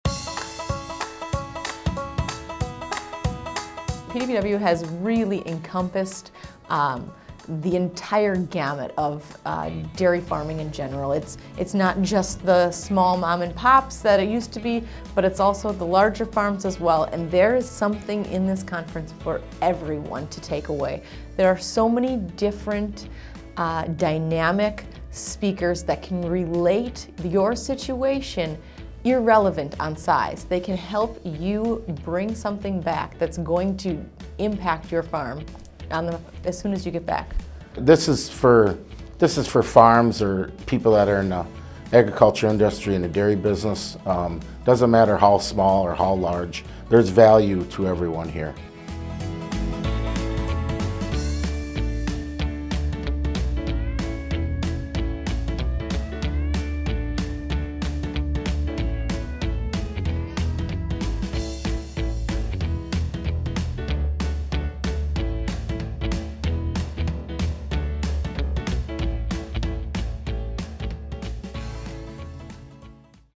Big or Small Dairy Farm - PDPW Business Conference is for You!
The Professional Dairy Producers (PDPW) annual Business Conference has offered best-in-class education and networking since 1993, when the producer-led group held their first Conference.

PDPW strives to provide education and inspiration for a vibrant and thriving dairy infrastructure, leaving a positive legacy for generations to come.

We bring together thousands of dairy farmers and agribusiness people from around the globe. This is dairy's go-to educational event providing keynote speakers, breakout sessions, hands-on labs, learning lounges and preview stages with the dairy industry's top experts. With 70 percent of attendees representing dairy farm owners and managers, you are exposed to the movers and shakers in dairying. Who else attends?  Food companies, marketers, distributors and other food system professionals that rely on the continuous improvement and viability of a strong dairy sector.

Business Conference is all inclusive. You'll have every opportunity to talk with a fellow producer over lunch, rub elbows with agribusiness leaders over the Connection "Wisconsin-Style Cheese, Beer and Wine" Reception, or take in the quiet of your thoughts or the music of the band. This is your time to shine; meet new people and make connections that will shape your profession for a lifetime.

It's all under one roof!  Alliant Energy Center Exhibition Hall, 1919 Alliant Energy Center Way, Madison, Wis.